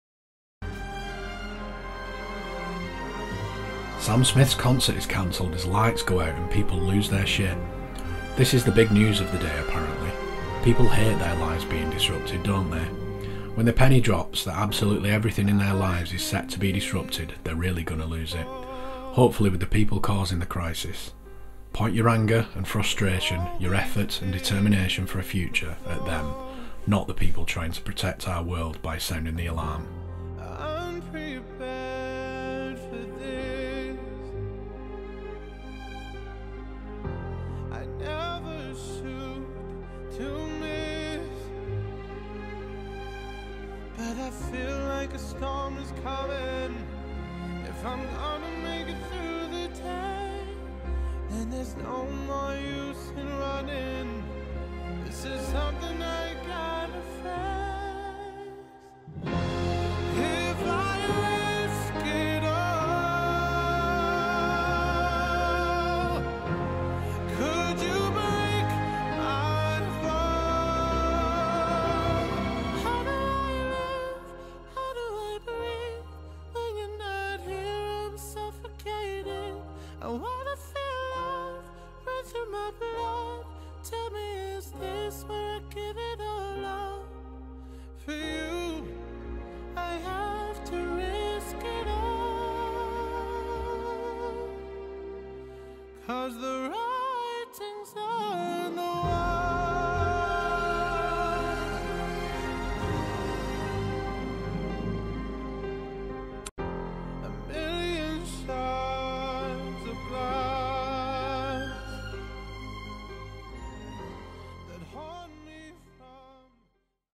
SAM SMITH CANCELLED
⏰ 10:30pm - SAM SMITH CANCELLED ❌
Sam smith's concert is cancelled as lights go out and people lose their shit, this is the big news of the day apparently. People hate their lives being disrupted don’t they. When the penny drops that absolutely everything in their lives is set to be disrupted they’re really going to lose it… hopefully with the people causing this crisis. Point your anger and frustration, your effort and determination for a future at them, not the people trying to protect our world by sounding the alarm.

It’s about how it feels once you know, when you are fully aware of the crisis after connecting emotionally.
It’s another attempt to communicate the urgency - This clip has been taken from the Full Film.

-- I’ve been working on this since May.
-- Grab a brew & enjoy 36:23 minutes of how I see the world now I know the full extent of the global crisis we face.
-- Spot the film clip, comedy character or music you may recognise as I take you through my thoughts on the 24th of May 2023.
-- Another attempt to get this urgent situation communicated.